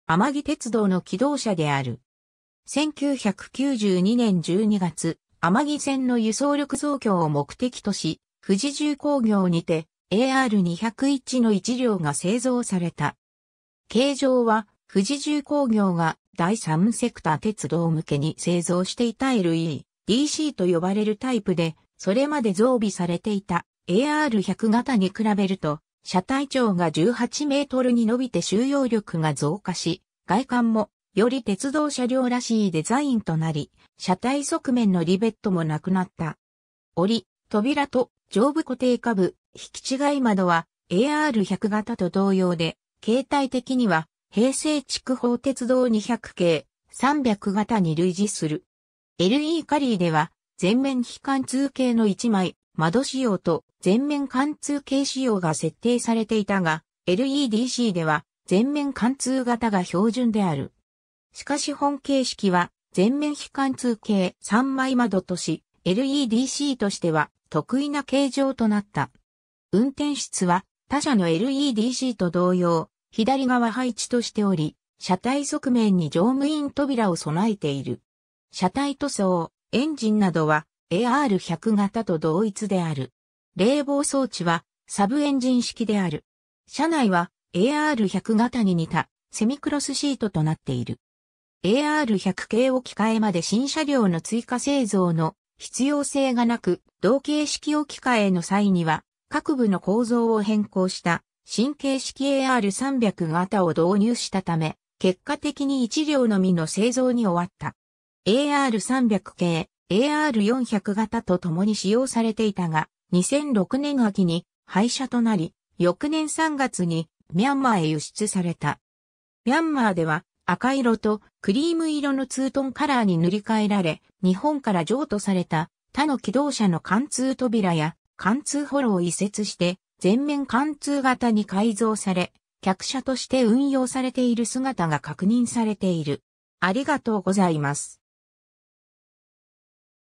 甘木鉄道AR200形気動車
甘木鉄道AR200形気動車（あまぎてつどうAR200がたきどうしゃ）は、甘木鉄道の気動車である。
 1992年（平成4年）12月、甘木線の輸送力増強を目的とし、富士重工業（現・SUBARU）にてAR201の1両が製造された。
 形状は富士重工業が第三セクター鉄道向けに製造していた　LE-DCと呼ばれるタイプで、それまで増備されていたAR100形に比べると、車体長が18 mに伸びて収容力が増加し、外観もより鉄道車両らしいデザインとなり、車体側面のリベットもなくなった。
折扉と上部固定下部引き違い窓はAR100形と同様で、形態的には平成筑豊鉄道200形・300形に類似する。
 LE-CarIIでは前面非貫通形の1枚窓仕様と前面貫通形仕様が設定されていたが、LE-DCでは前面貫通形が標準である。
しかし本形式は前面非貫通形3枚窓とし、LE-DCとしては特異な形状となった。
運転室は他社のLE-DCと同様、左側配置としており、車体側面に乗務員扉を備えている。
 車体塗装・エンジンなどはAR100形と同一である。
冷房装置はサブエンジン式である。
車内はAR100形に似たセミクロスシートとなっている。
 AR100形置き換えまで新車両の追加製造の必要性がなく、同形式置き換えの際には各部の構造を変更した新形式AR300形を導入したため、結果的に1両のみの製造に終わった。
 AR300形・AR400形とともに使用されていたが、2006年（平成18年）秋に廃車となり、翌年3月にミャンマーへ輸出された。
ミャンマーでは赤色とクリーム色のツートンカラーに塗り替えられ、日本から譲渡された他の気動車の貫通扉や貫通幌を移設して前面貫通形に改造され、客車として運用されている姿が確認されている。